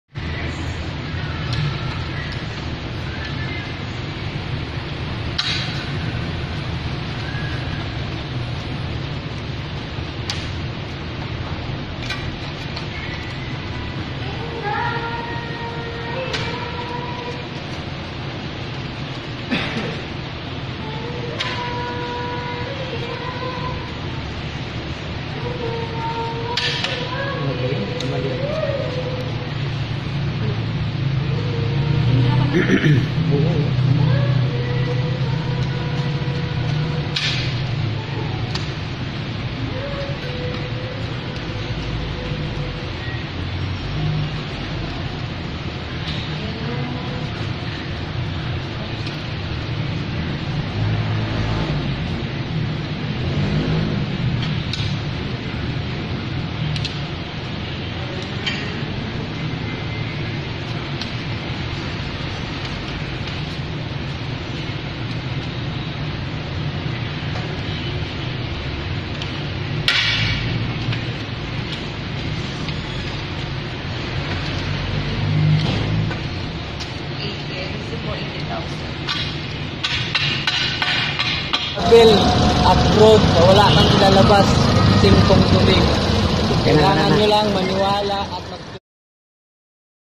Suzuki Avenis 125 Ang Nabili dahil sa Online Business Ng RF3 WORLD
Nakabili ng motor Dahil sa Online Business ng RF3 WORLD, Gusto nyo Rin ba? Be my Business Partner Dito sa RF3 WORLD turuan Kita...